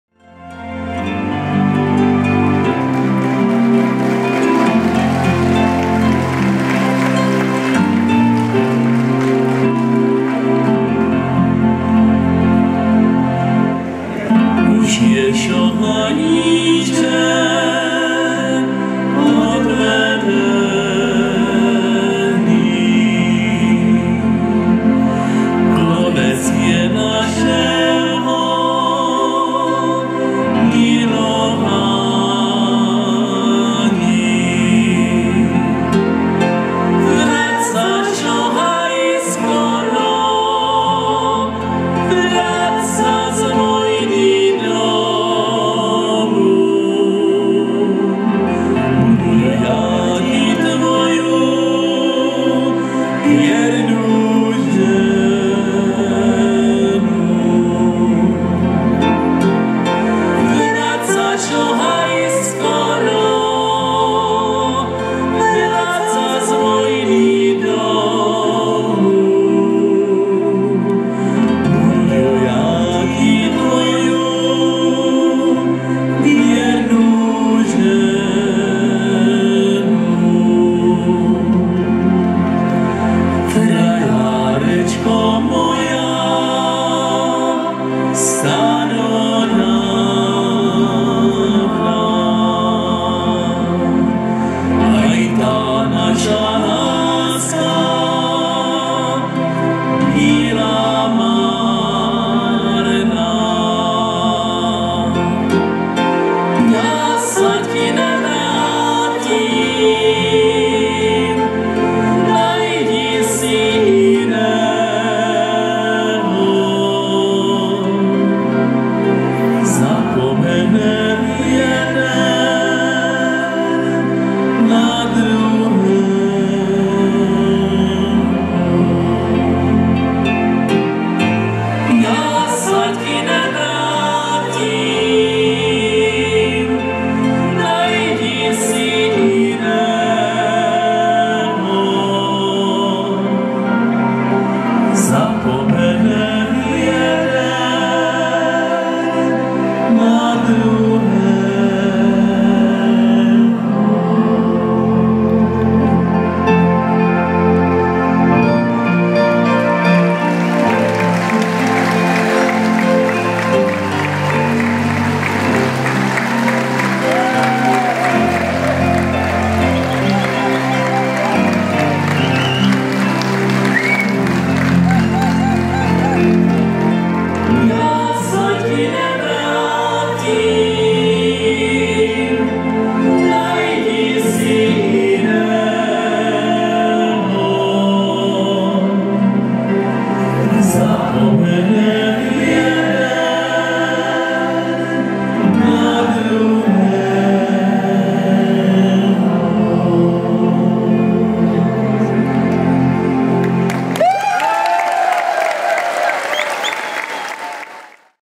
SOUTĚŽ:Kterou pěveckou dvojici z TV ŠLÁGR by jste vybrali za nejlepší? O.BARAČIČOVÁ a Š.MACHALA
-prosím pište, klikejte: líbí se
-když napíšete i svoji adresu, budete vylosováni a obdržíte CD těchto zpěváků